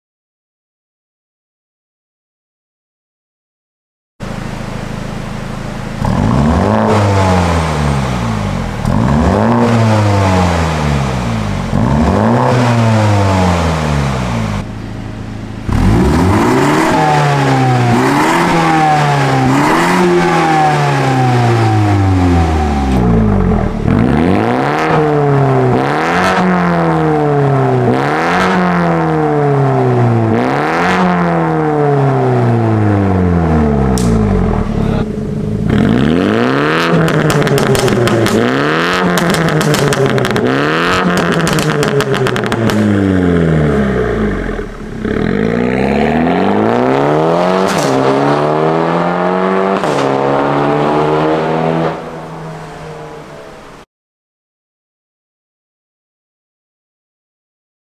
BMW F40 M135i Sound Test w/ Valvetronic Catless Fi EXHAUST X Utmost Downforce Garage
BMW F40 M135i is installed with Fi EXHAUST valvetronic catback, catless downpipes and dual tips in silver for increased performance and sound.